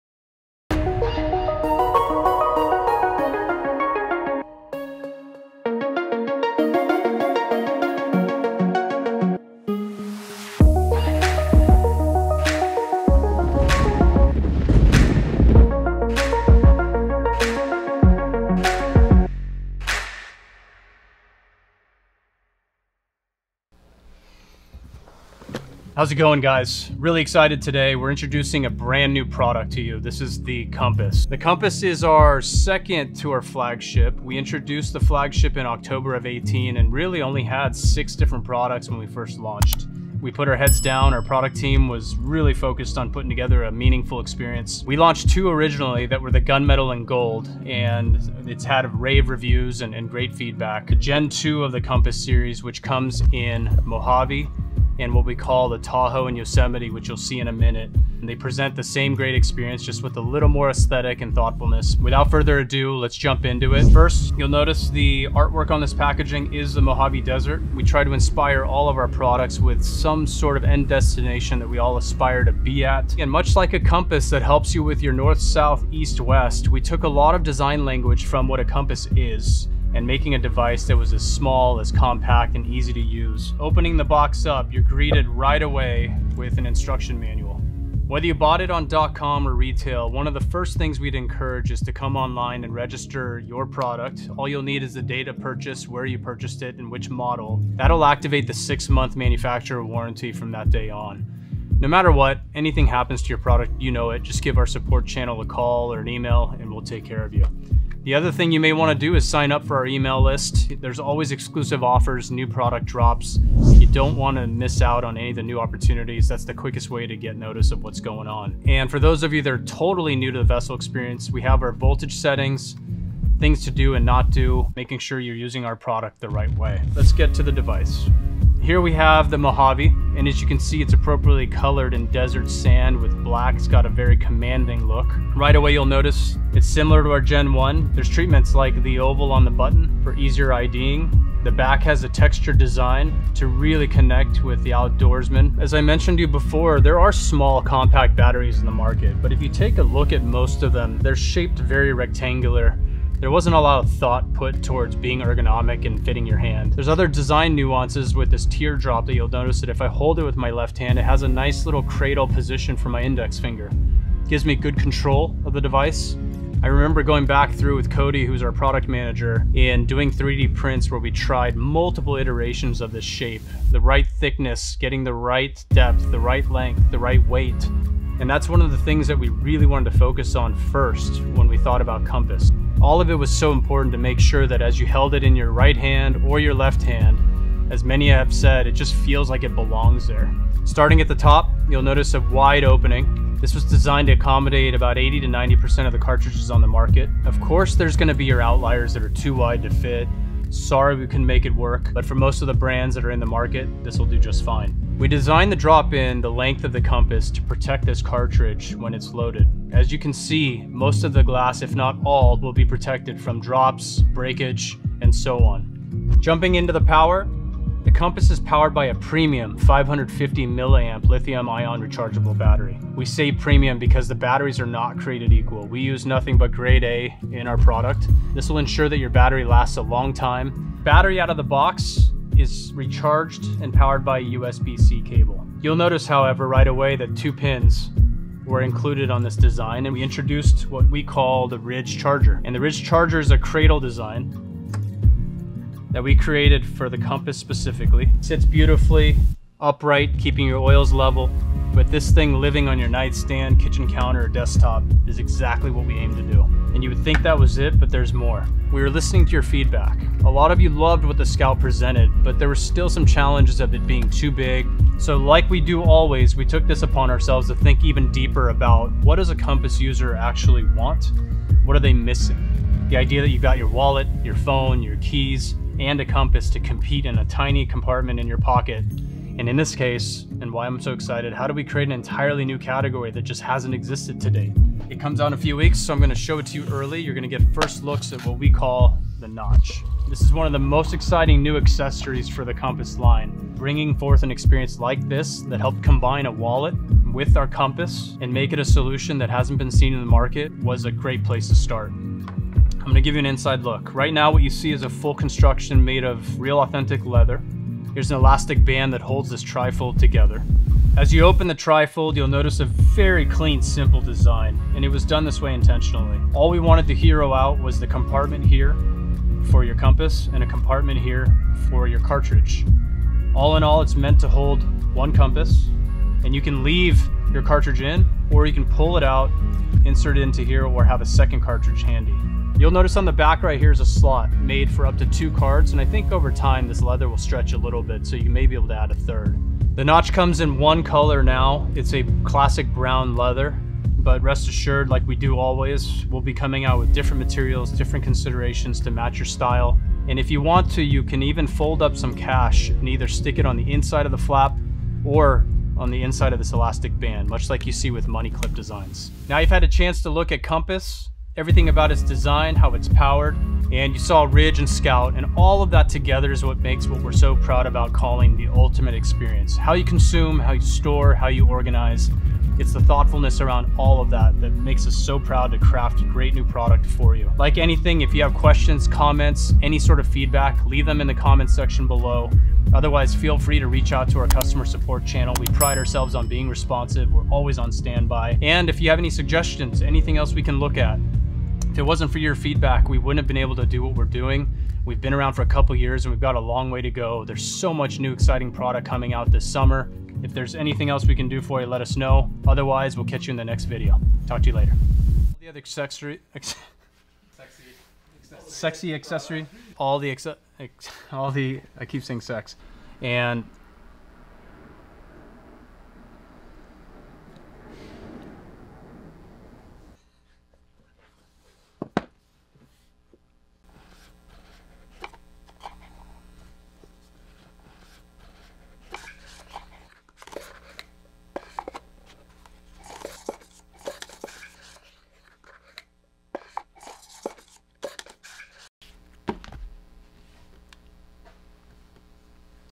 Introducing Compass || Why We Strive To Be BETTER
Introducing Compass. We welcomed the challenge of designing a small, yet powerful device that takes form and function to new heights. The Compass features an incredible high-capacity battery, our signature tuned airflow and an unparalleled ergonomic design that feels like it belongs in your hand.

Available in Gold, Mojave, Yosemite, and Tahoe.

+  Features a protected, cartridge drop-in design
+  Fits most 510 thread cartridges (cartridge not included)
+  Features cartridge swivel for proper mouthpiece alignment
+  Powered by a high-quality 550mAh lithium-ion rechargeable battery core
+  3-LEDs indicate battery life and power settings
+  Four (4) power/voltage settings (2.4V, 2.8V, 3.2V, 3.6V)
+  USB-C charging port + cable
+  Dimensions: 2.625"L x 1.625"W x 0.75"D
+  Weight: 66g
+  Six (6) month limited warranty
+  Compatible with Ridge Charger and Scout Cases